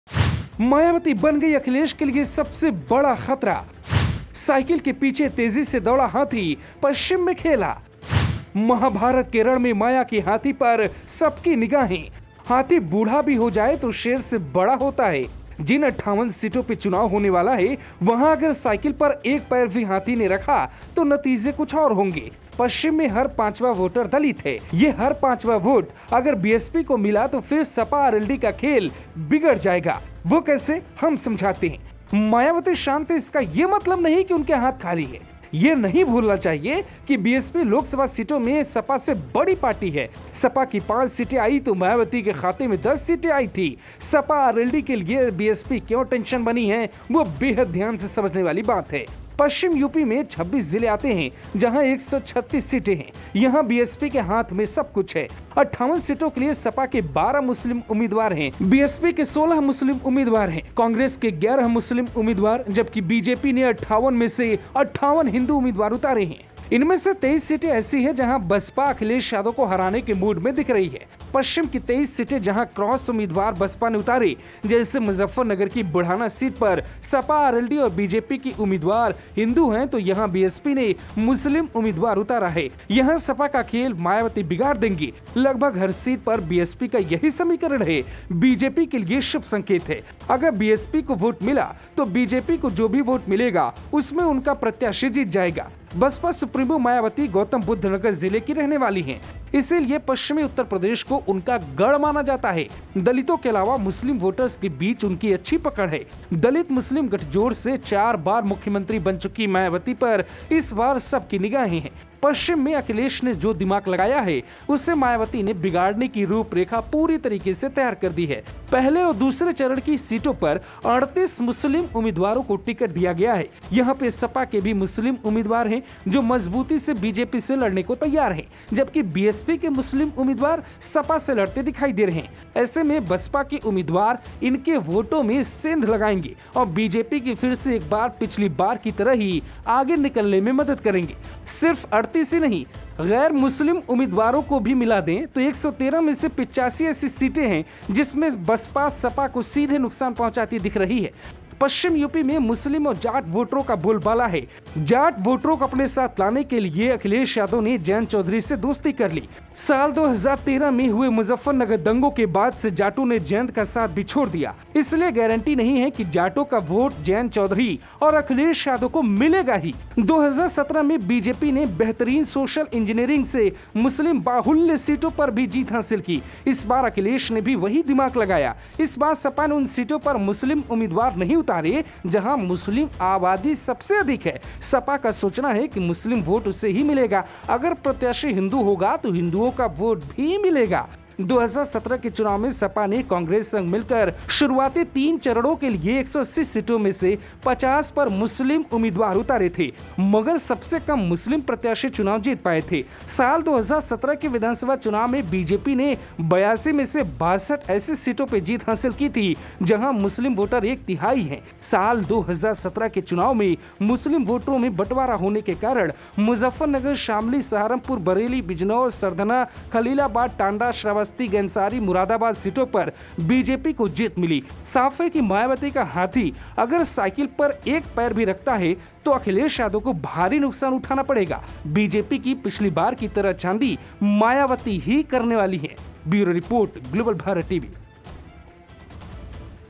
हाथी, साइकिल के आगे आ रहा है हाथी ने साइकिल पर एक पांव भी रखा तो सपा के 58 सीटों का गणित बिगाड़ देगा
हाथी बुढ़ा भी हो जाए तो शेर से बड़ा ही होता है...जिन 58 सीटों पर चुनाव होने वाला है...वहां अगर साइकिल पर एक पैर भी हाथी ने रखा दिया तो नतीजे कुछ और होंगे...पश्चिम में हर पाचवां वोट दलितों का है...ये हर पाचंवां वोट अगर BSP को मिली तो फिर सपा-RLD का खेल बिगड़ जाएगा...वो कैसे हम समझाते है.